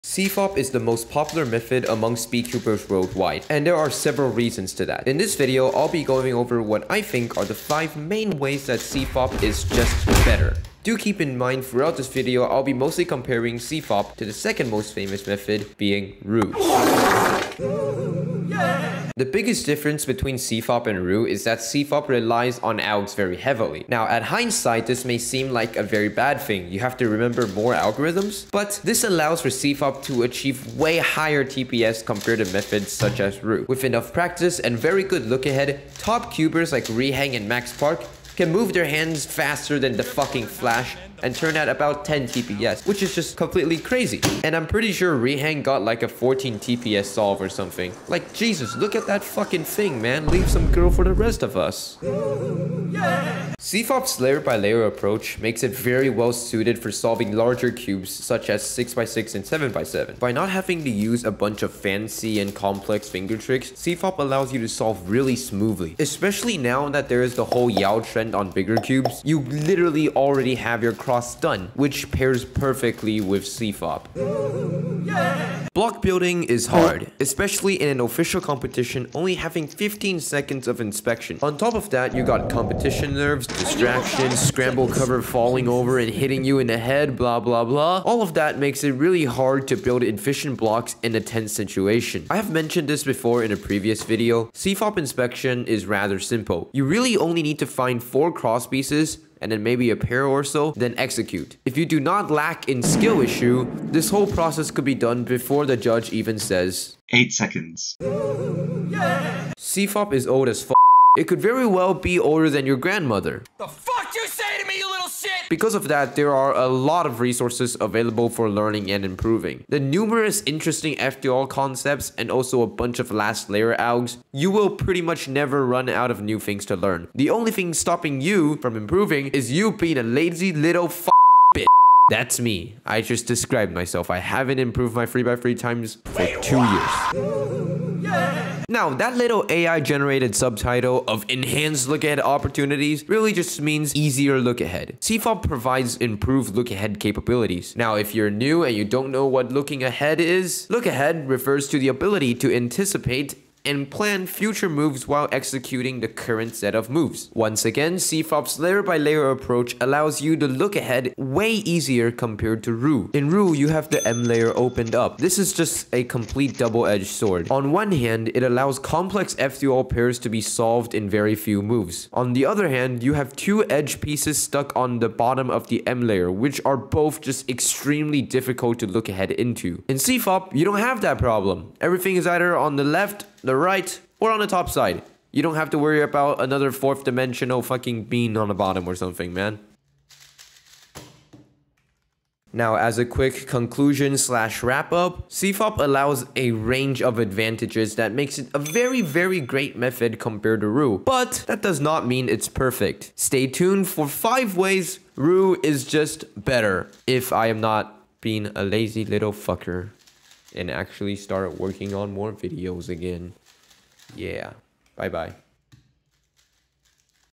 5 Ways CFOP is AMAZING!!
Dumb video.

Hello there! After uploading the video, I think I didn't really make the my intentions clear, This video is not to talk bad or downplay roux, this video is to highlight on what makes CFOP GOOD and that other methods not do so well at a particular point. (I only used Roux because its the second most popular method and its what people are most familiar with other than CFOP!).

Mb for at some points it seems like I'm saying roux is bad, but no it was completely not the intent. If you look at my channel a bit more, I USE Roux and have made a video trying to help some ppl just starting out trying to switch to it thru my personal experience. It's totally fair if anyone disagrees with my points in the video or thinks its a bad video.

With all that said the most important thing, is def don't take the vids too seriously, its really just my opinion thru my experience sharing my thoughts, using a joking tone and other stuff to maybe at least make a few people laugh :0

On an extra note to clear up some things:
- 0:32 Some ppl think its me being bad that I claim "CFOP with more algs allow you to have higher than roux" but I think its widely agreed upon that cfop does have higher tps than roux, but its not like roux needs the tps lol. Its significantly more effective (move count wise) than CFOP.

- 1:15 I said block building is hard, yes its my skill issue, but thats what I experienced from using roux in OH in comp, I find it harder than CFOP which have 4 very obvious pieces and a set location to go, While in roux you have so many options and comp nerves makes it really hard to find a effective and good FB for me.

- At 1:38 I said 6 pieces is needed to make FB because
a) some of the time you'll actually be moving the centers (with things like S moves or E moves.) With CFOP yes there are Uw moves that move the cross, but its all in a order you would already know, and it is a lot less confusing since its has less freedom than roux does (like you mostly would either be aligning center to cross OR cross to center)

b) if you went the other video I was talking about (being I tried to point out (in the inspection part of the video) that lookahead into first pair while doing cross on CFOP is a lot easier to do than lookahead on a square into a full FB. So to at least get the same times on roux you should at minimum be able to fully inspect FB imo.

-I never said that Roux doesn't have resources, I was simply stating "CFOP has a lot of resources", I wasn't trying to make a comparison

- CFOP is just better for big cubes (that was simply my statement), thats why I didn't even mention roux in that part of the video.

- The title was title was clickbate, and a bit misleading and kinda rubbing some ppl the wrong way, so that has been changed.

I really appreciate suggestions and discussions but I don't think coming in from a point just calling the video terrible is constructive criticism and doesn't really help me understand why/how the vid is trash.

anyways have a great rest of yo day!